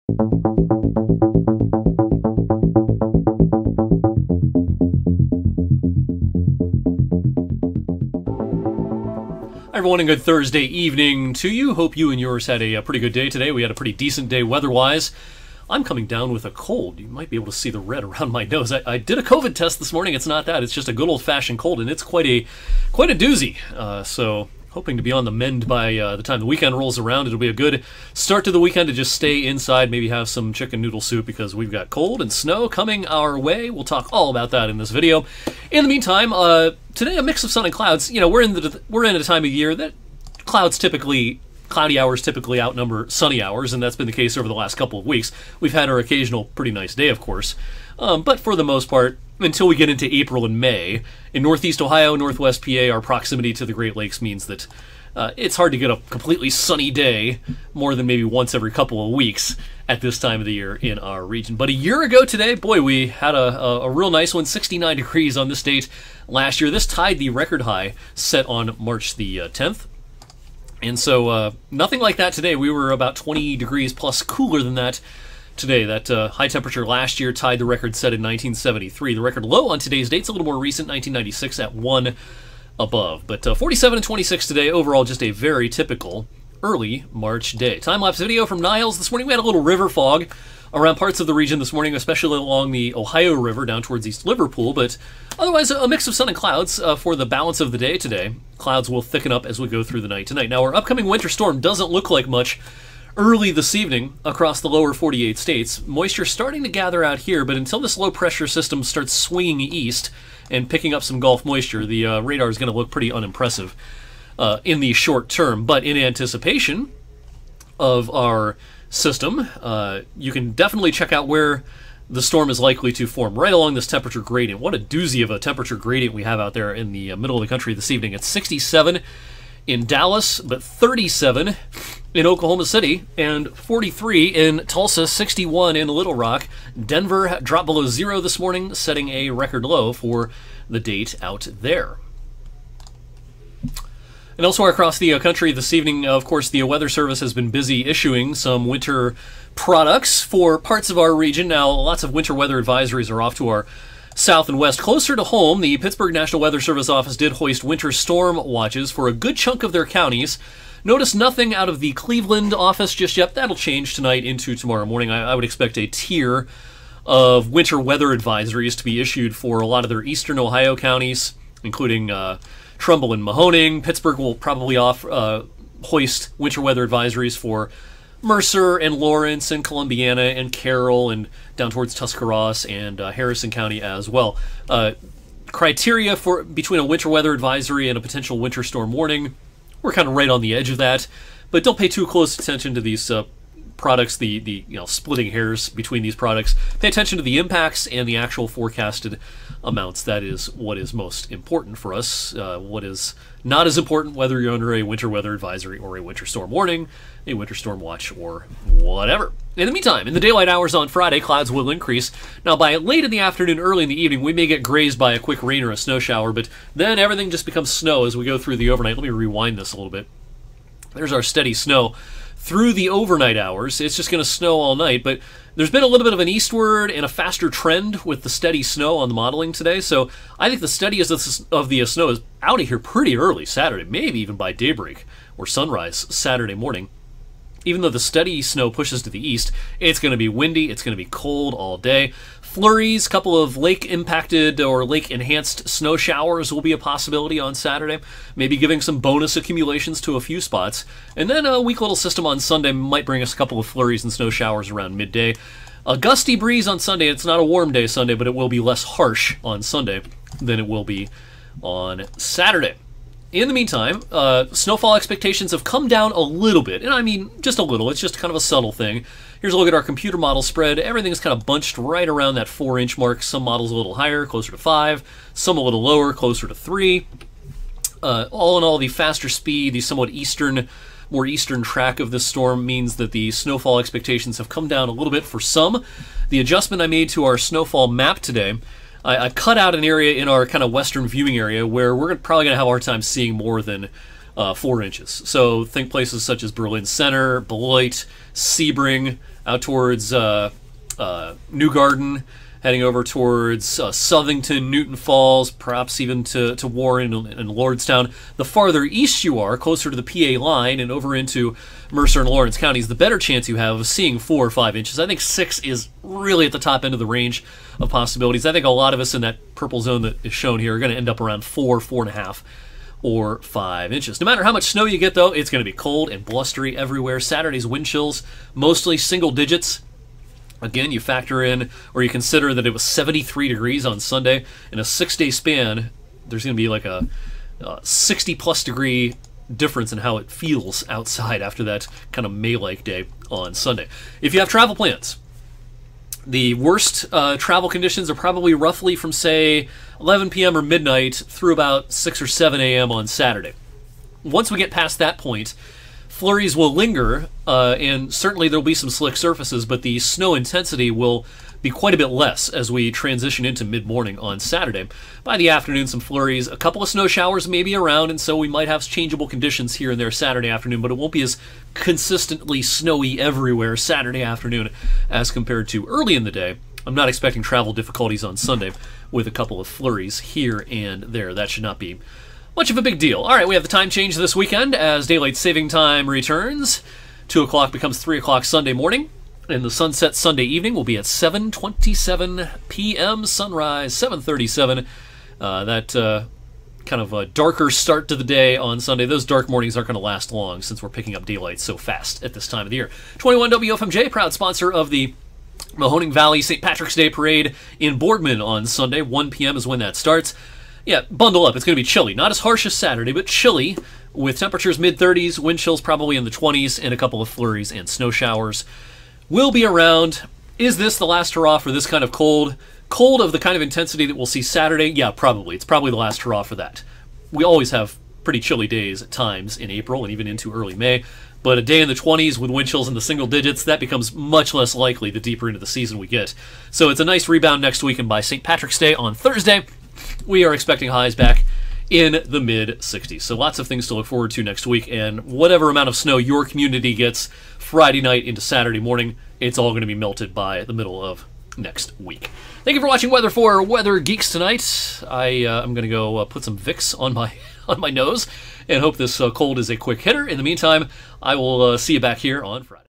A Quick-Hitting March Snow Blitz: "Weather For Weather Geeks" 3/10/22
00:00-00:34 Intro
00:34-01:37 A Look Back
01:37-03:17 Current Conditions
03:17-06:21 What To Expect With Snow, Cold, Wind
06:21-07:42 Sunday, Next Week